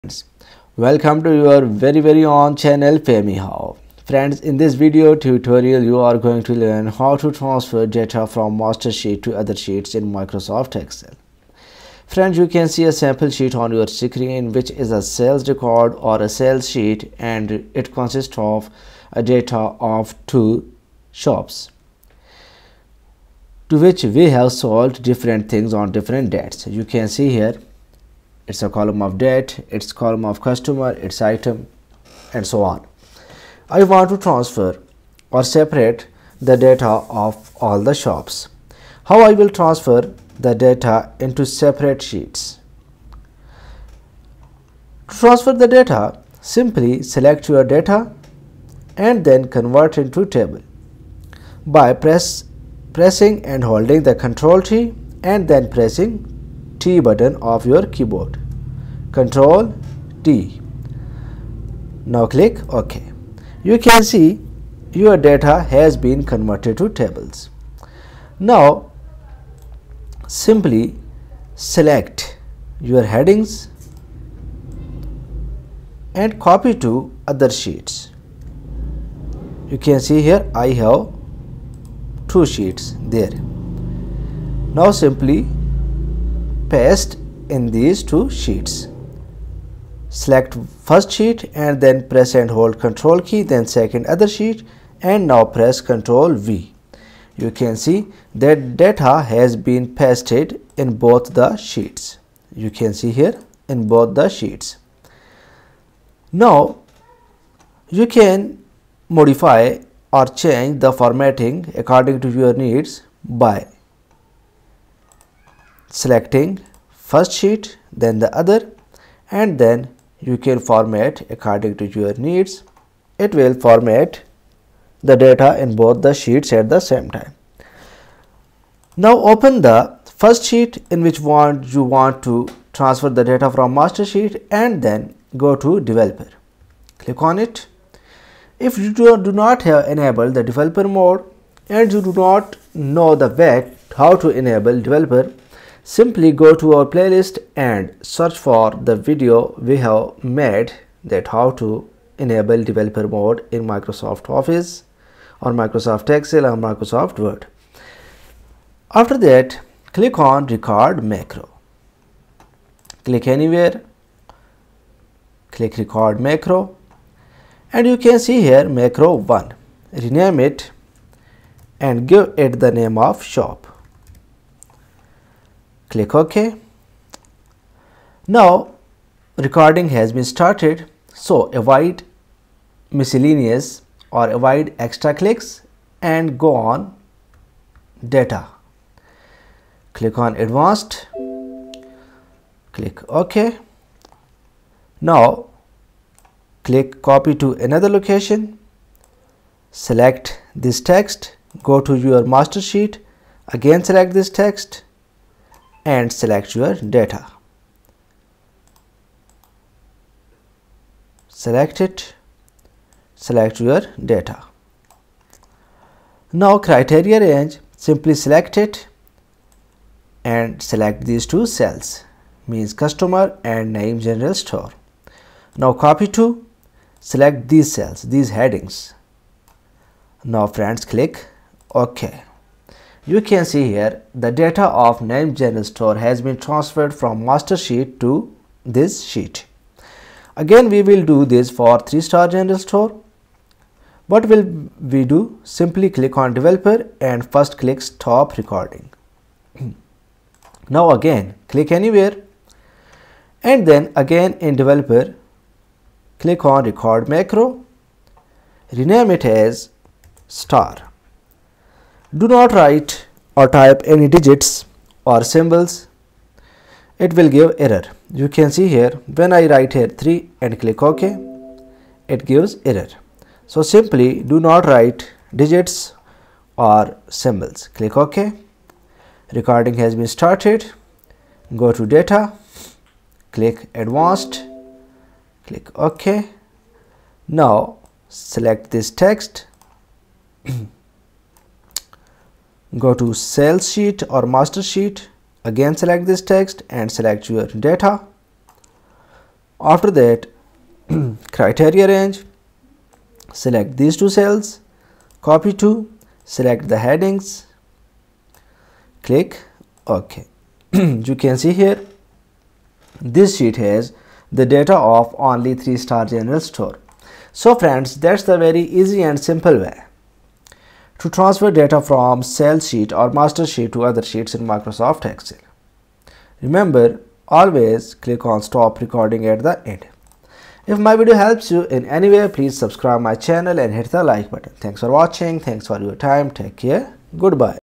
How to Transfer Data From Master Sheet to Other Sheets in MS Excel | Copy Data From Master Sheet
This video tutorial will show you the ways to transfer data from matter sheet to other sheets in MS Excel. After watching this video tutorial you will be able to transfer data from Master Sheet to Other Sheets in MS Excel.
This video tutorial may work in MS Excel 2007, 2010, 2013, 2016, 2019, 2021 and 365.